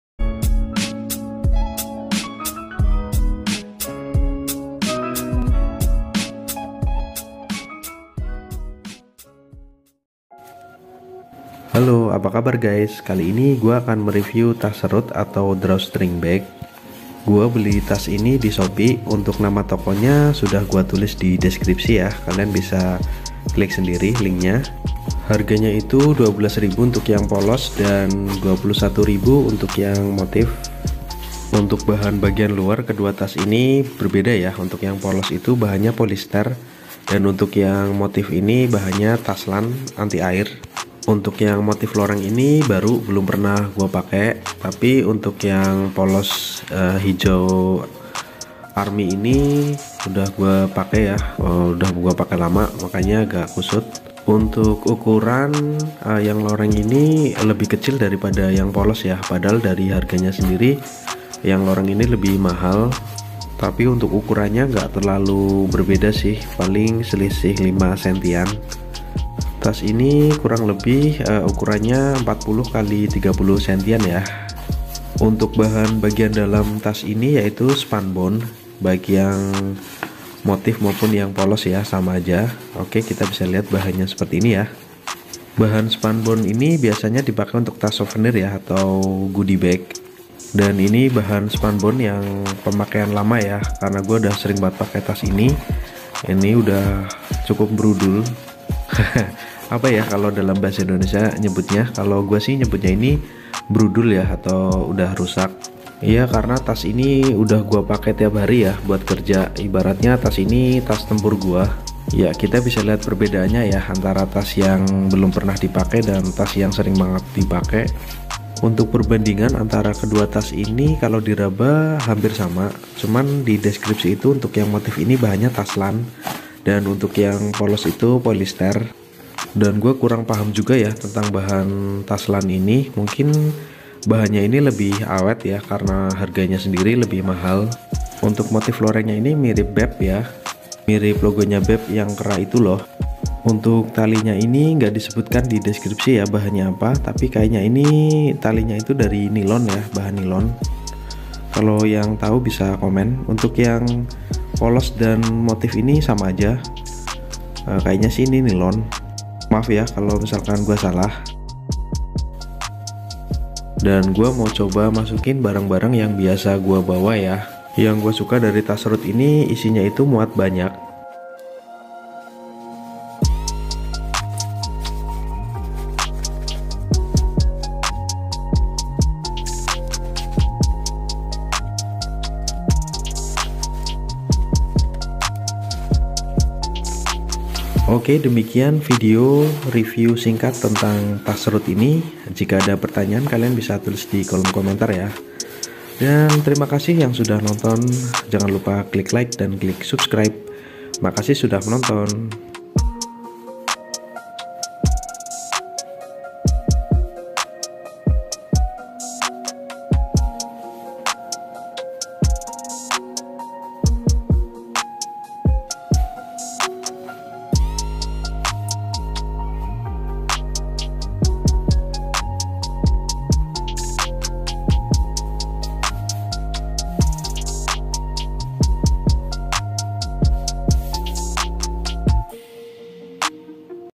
REVIEW TAS SERUT / DRAWSTRING BAG KEREN DAN MURAH BISA BUAT DI-RESELL
Kali ini gua akan me-review tas serut / drawstring bag. Tas serut ini menjadi tas andalan gua ketika pergi kemana aja. Karena tas-nya enteng, muat banyak, dan praktis dibawa kemana-mana. Tas serut ini juga murah banget, jadi cocok buat dijual lagi. Dan yang paling keren, tas serut ini cocok banget untuk penunjang fashion kalian, dijamin oke parah. Daripada penasaran yuk ditonton videonya.